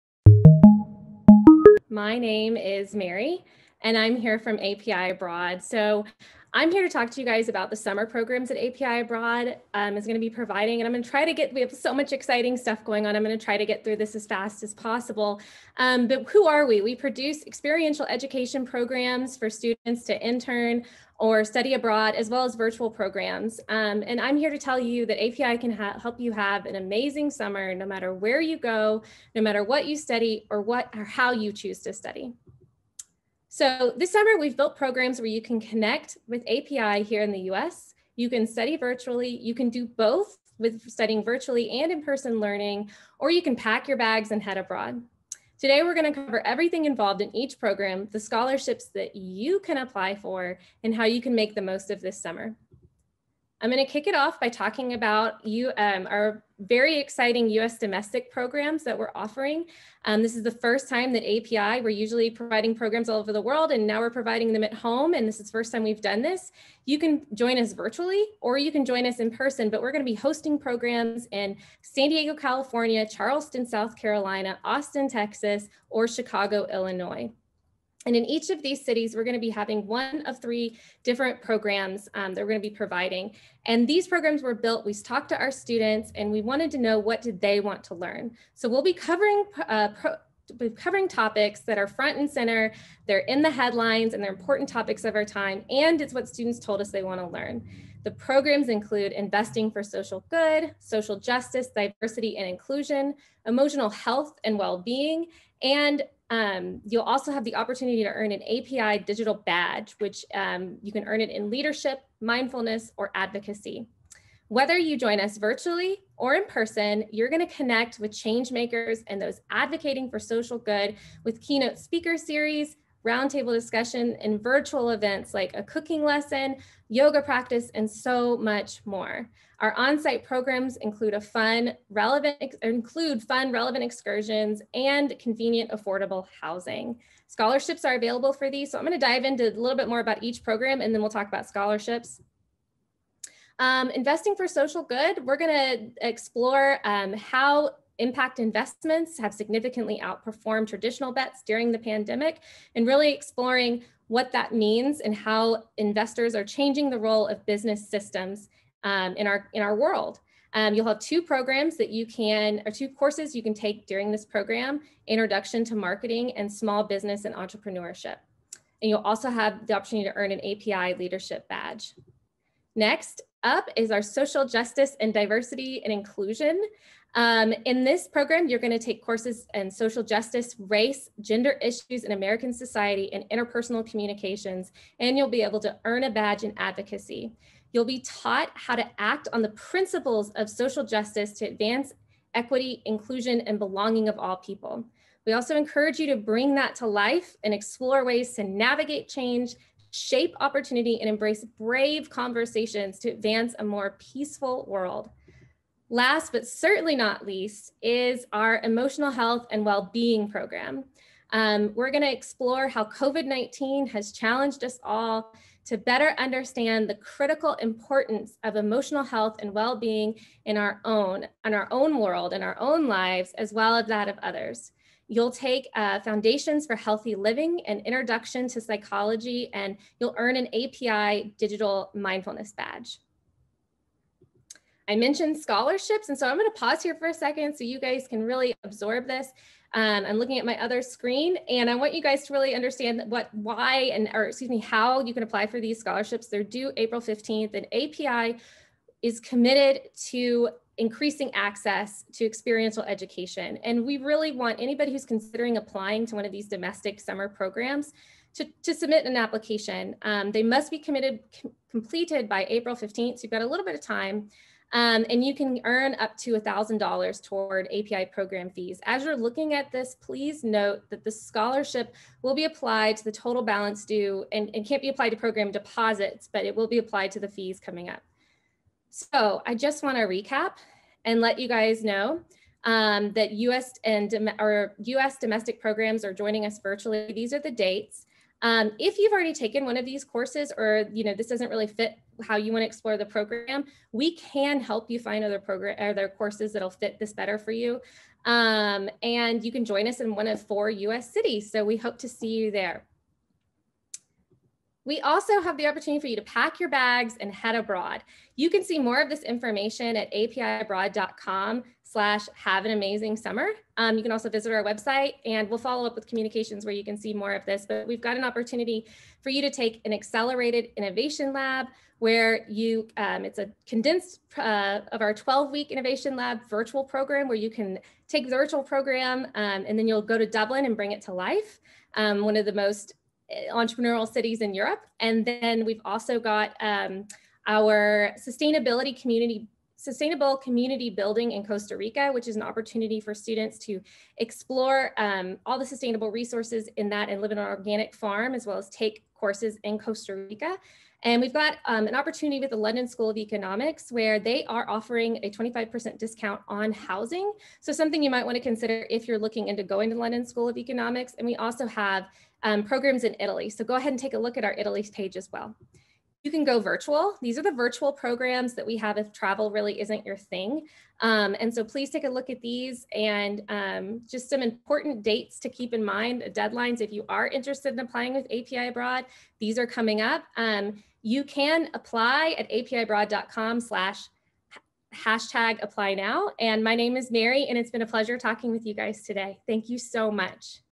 API Abroad: TeenLife LIVE Virtual Summer Opportunities Fair 3/25
In this clip from the TeenLife LIVE Summer Opportunities Fair on March 25, 2021, you can learn more about all the opportunities available to you with API Abroad